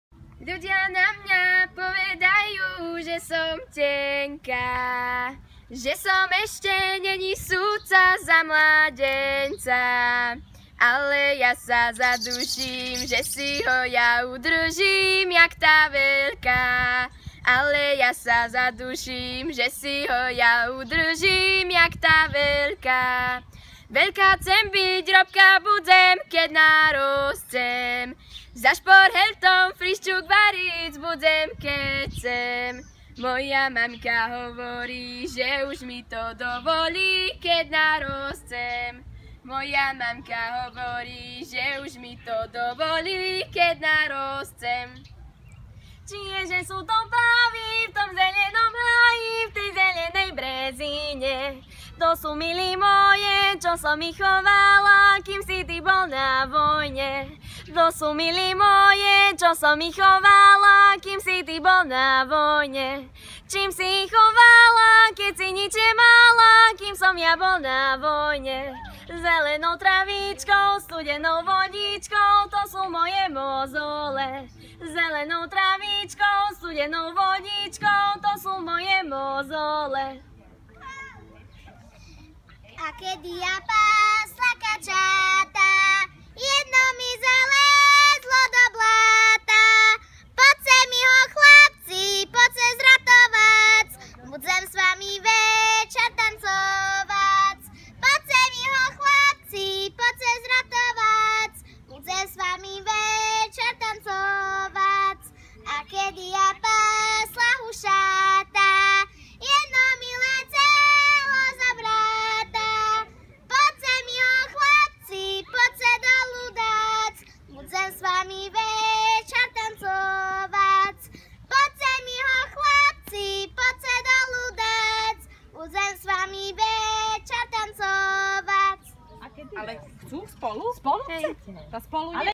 Slávik Slovenska 2016
Príjemným spestrením dnešného dňa mohol byť pre každého náhodného okoloidúceho spev ľudových piesní v Zuzkinom parku. Dôvod bol prozaický, v neďalekom Átrium klube sa odohrávalo okresné kolo v interpretácií ľudových piesní Slávik Slovenska 2016 pre žiakov základných škôl a osemročných gymnázií. Vychutnajte si speváčky v ľudových krojoch v našom videu.